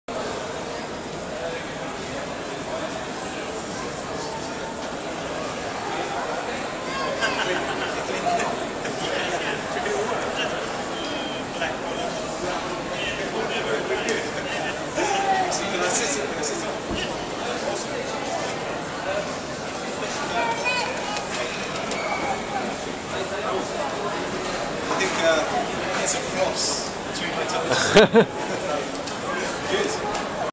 Vibram Fivefingers Shoe Shiner: Amused, bemused or confused?
While he was clearly amused at the prospect - and, indeed, offered to polish them black, we were left disappointed he wasn't fully equipped with dental floss...